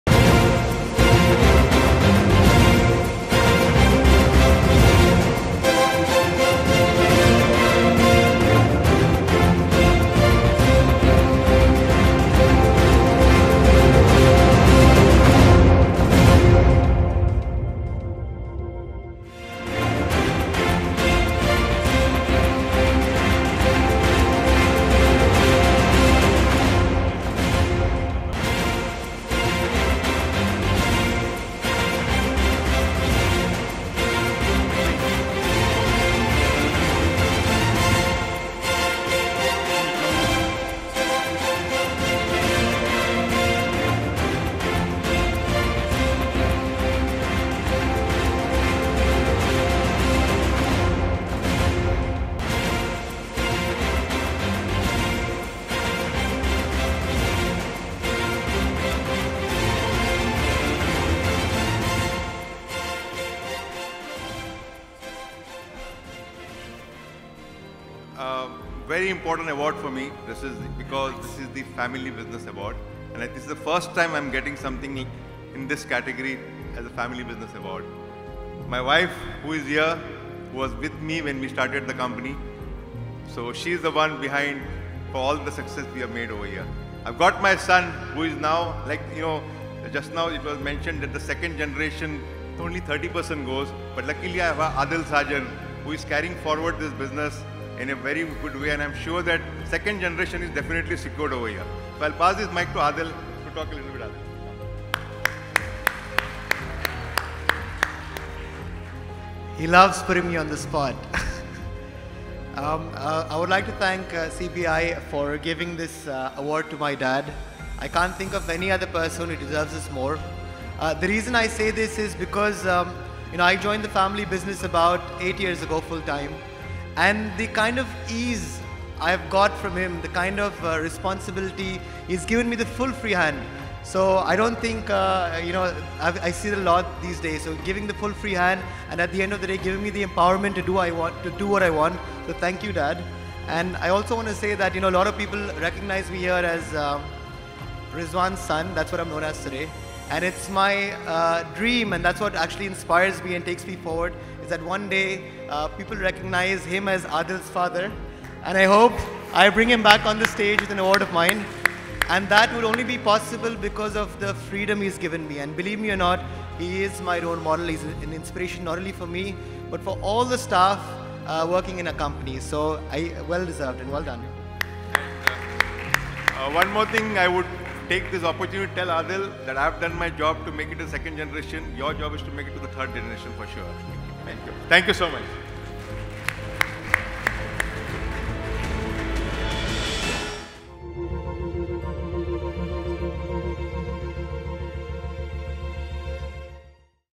Family Business Leader
The Stars of Business Leadership Award for the category "Family Business Leader of the Year" was awarded to Rizwan Sajan, Founder and Chairman, Danube Group.

What really stands out in Rizwan’s profile is the fast-paced growth of his company. In a short span of time, he’s achieved significantly more than industry veterans. More importantly, he has slowly and strategically led his company to become the market leader in the price segment which he operates. He has created this niche for himself and that has propelled him to new heights. What makes him suitable for this category is that his family members have been a core part of his team from the very get-go; building the company’s ethos with him from Day One.